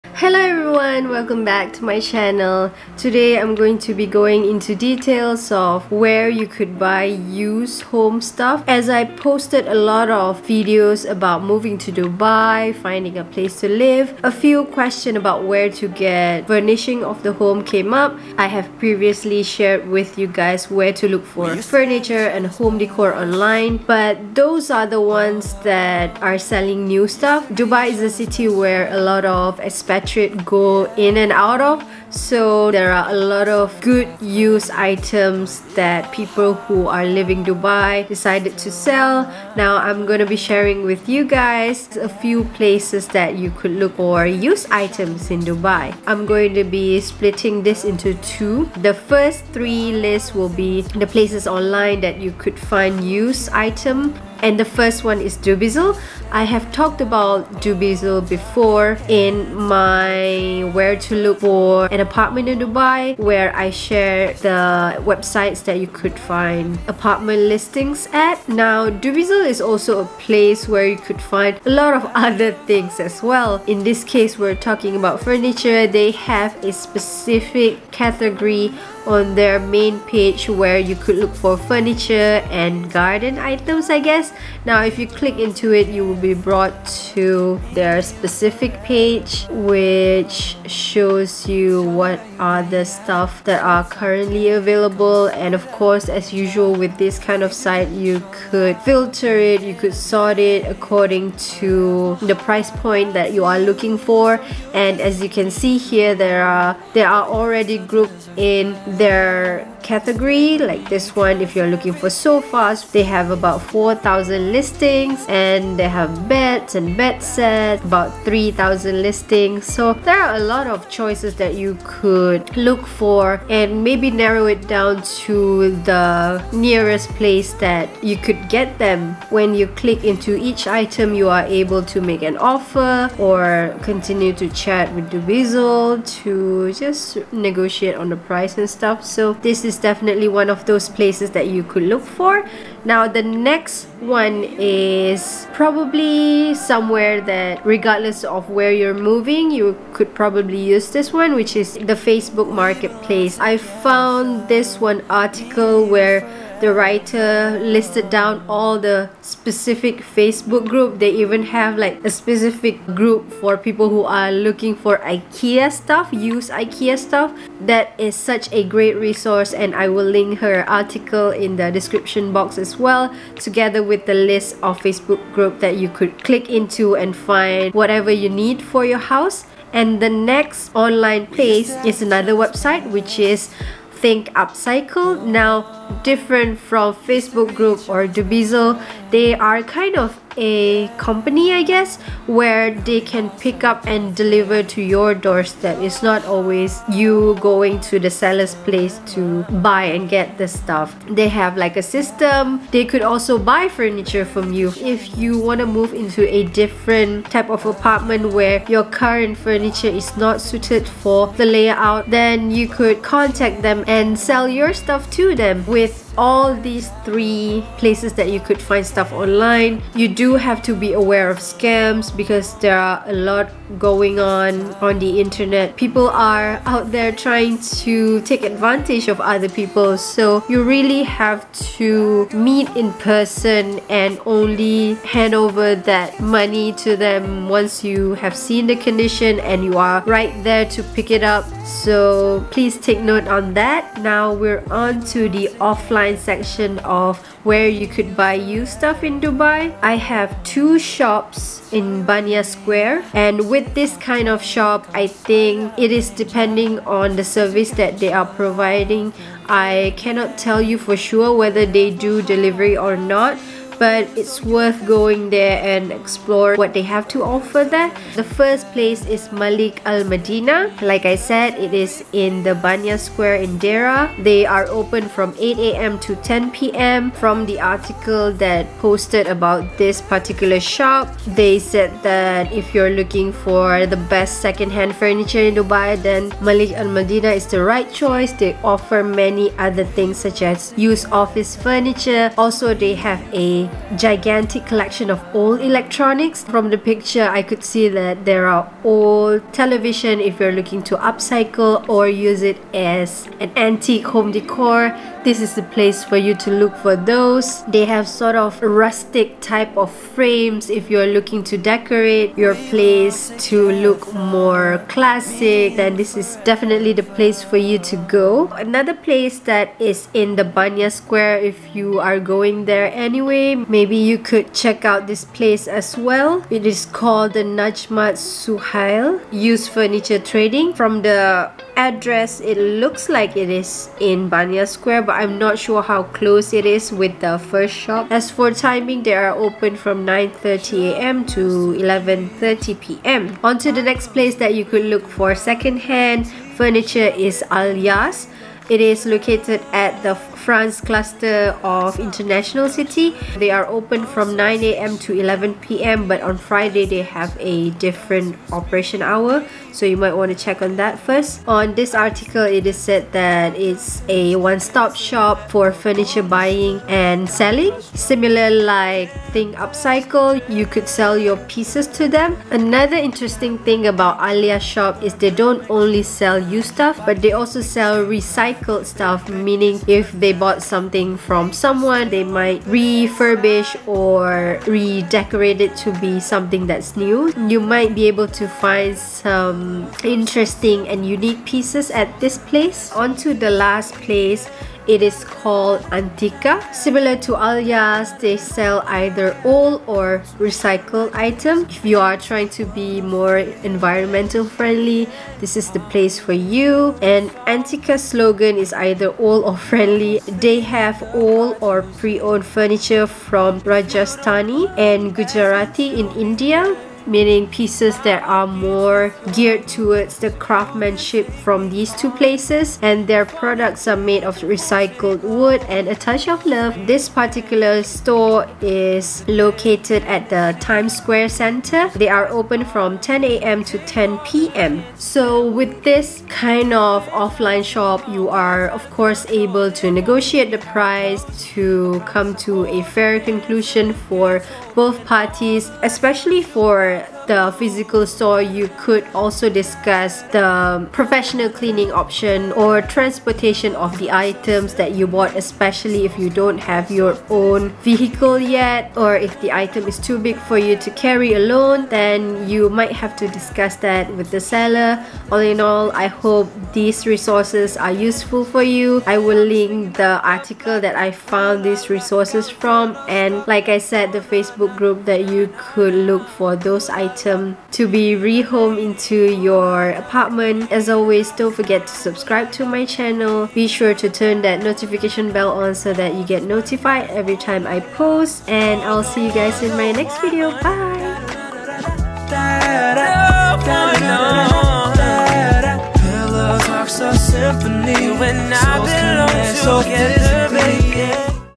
DUBAI SECONDHAND FURNITURE | where to get used furniture and home appliances in dubai
♥ MENTIONED PLACE ♥

Facebook Marketplace
- Dubai || Sharjah Buy Sell Second Hand Marketplace

Malik Al Madina
Baniyas Square, Deira.
08:00 am to 10:00 pm

Najmat Suhail
Al Nakhal Street, Behind Melody Queen Hotel, Baniyas Square, Deira.
09:30 am to 11:30 pm

Alyas
France Cluster, International City.
Saturday to Thursday: 09:00 am to 11:00 pm | Friday: 04:00 pm to 11:00 pm

Antika
Times Square Center.
10:00 am to 10:00 pm

(sometimes I use ES too!)

Fadia